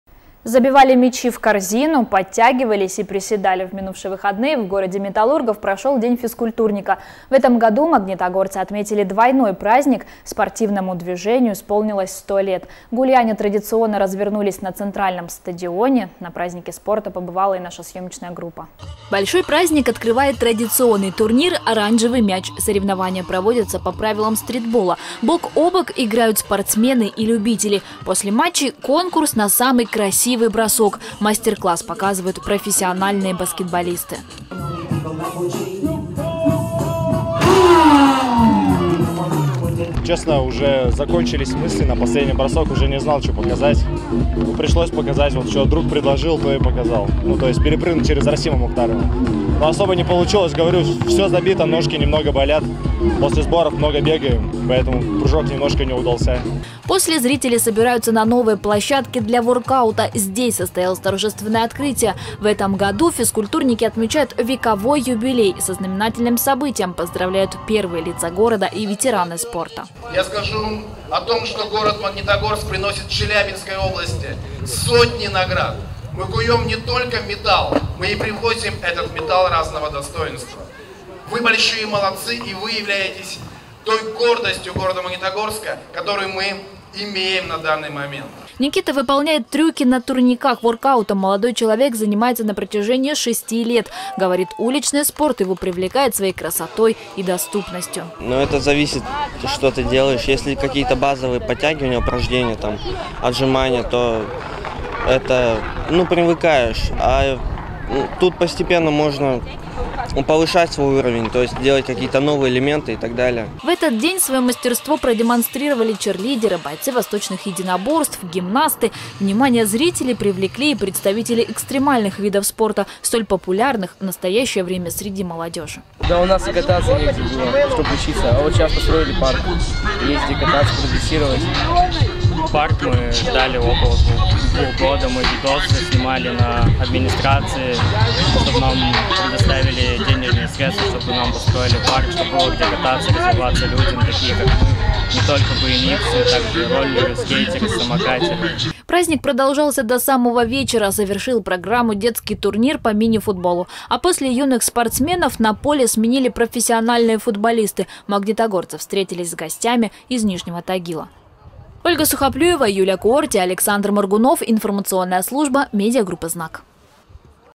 ПРАЗДНИК СПОРТА
Забивали мячи в корзину, подтягивались и приседали. В минувшие выходные в городе металлургов прошел День физкультурника. В этом году магнитогорцы отметили двойной праздник. Спортивному движению региона исполнилось 100 лет. Гулянья традиционно развернулись у Центрального стадиона.